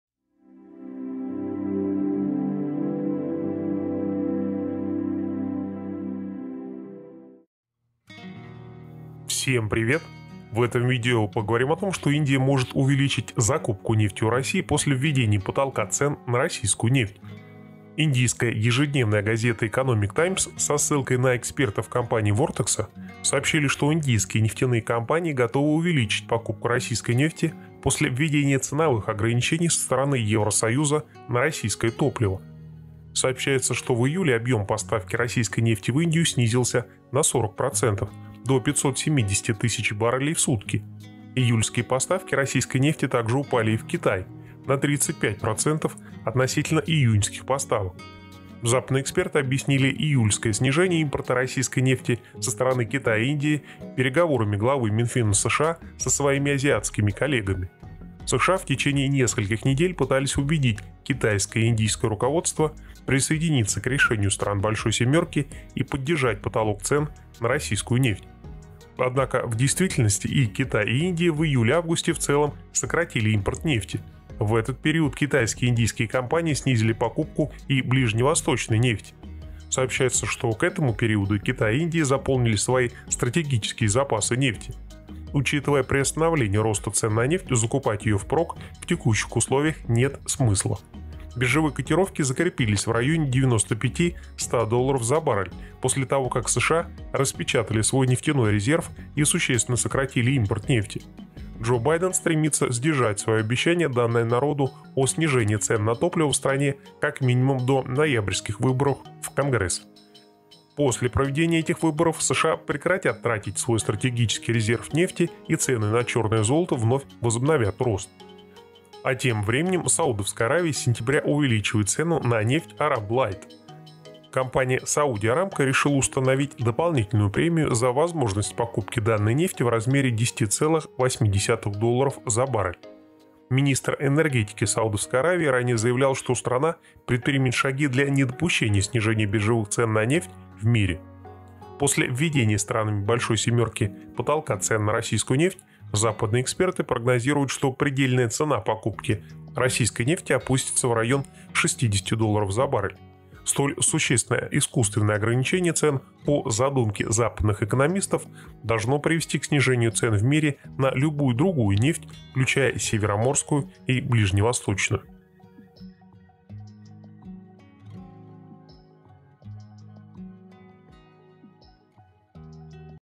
Индия может увеличить покупку нефти у России после введения потолка цен на российскую нефть
Индия может увеличить покупку нефти у России после введения потолка цен на российскую нефть. Индия увеличит импорт российской нефти после введения ценовых ограничений странами G7. Индия сократила покупку российской нефти в июле и августе 2022 года. Индия и Китай продолжат покупать российскую нефть на прежних условиях